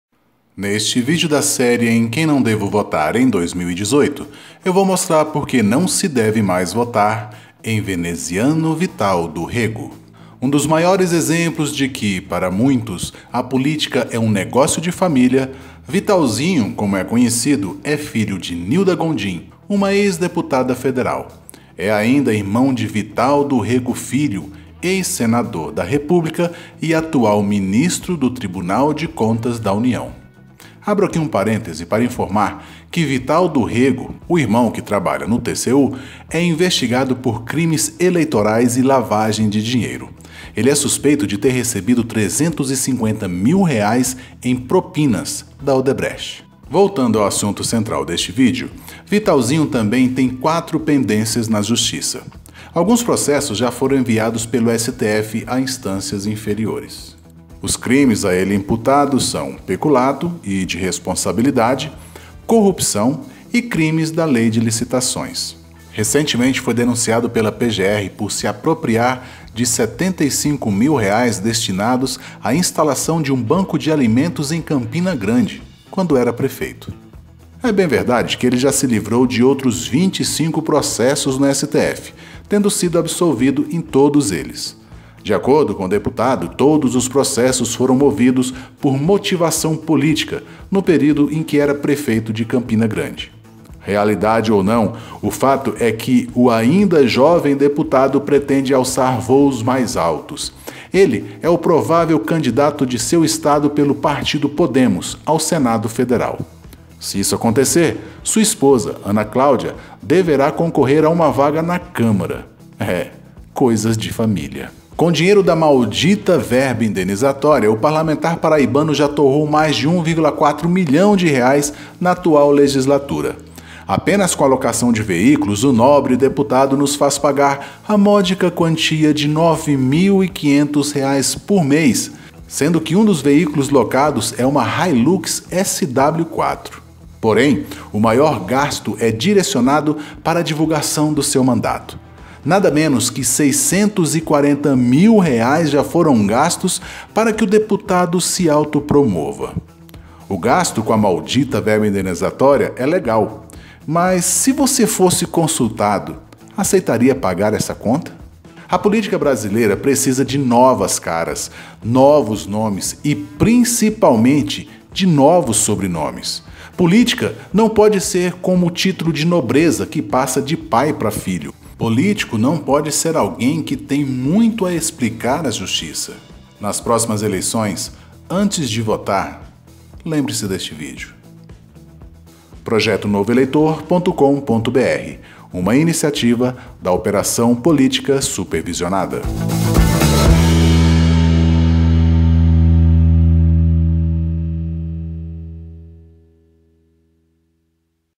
"Em quem não devo votar em 2018" - Veneziano Vital do Rêgo
Um dos maiores exemplos de que, para muitos, a política é um negócio de família, Vitalzinho, como é conhecido, é filho de Nilda Gondim, uma ex-deputada federal. É ainda irmão de Vital do Rêgo Filho, ex-senador da República e atual ministro do Tribunal de Contas da União.

Como fiscalizar contas públicas do seu município (Tutorial OPS)